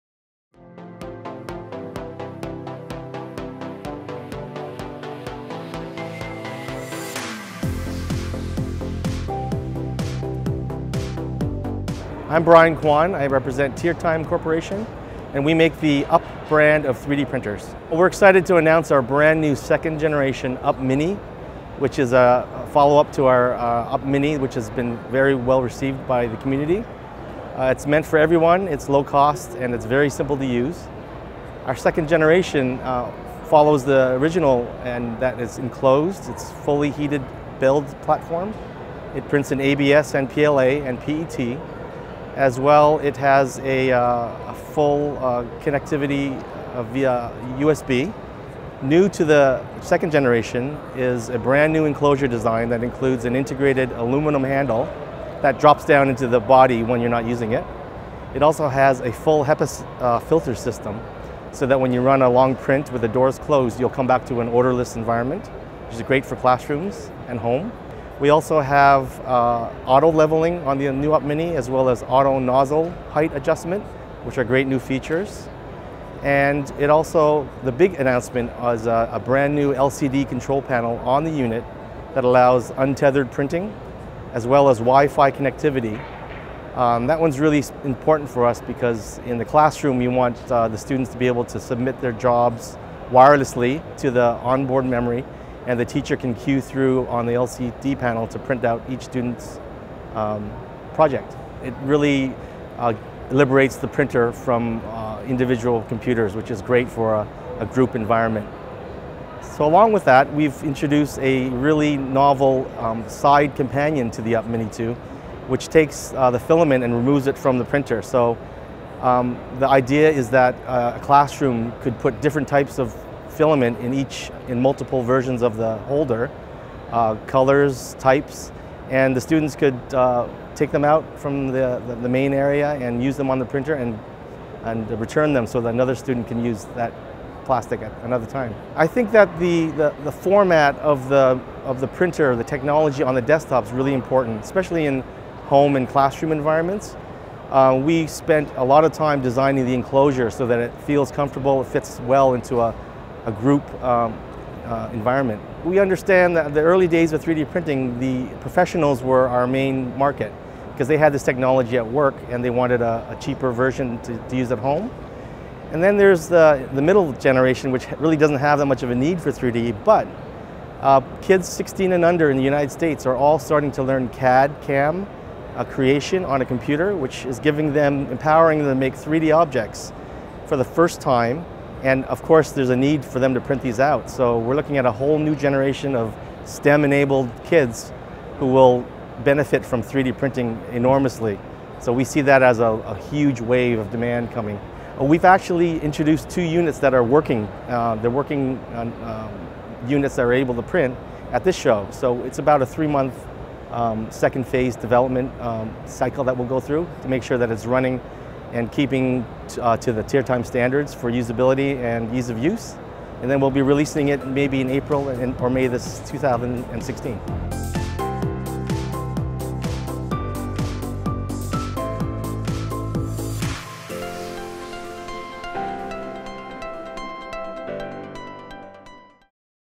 Tiertime on the second generation Up mini 2 3D printer at CES 2016
Brian Quan gives TCT the lowdown on Tiertime's next-generation UP mini 3D printer designed for the home and a whole new generation of STEM-enabled students in the classroom environment.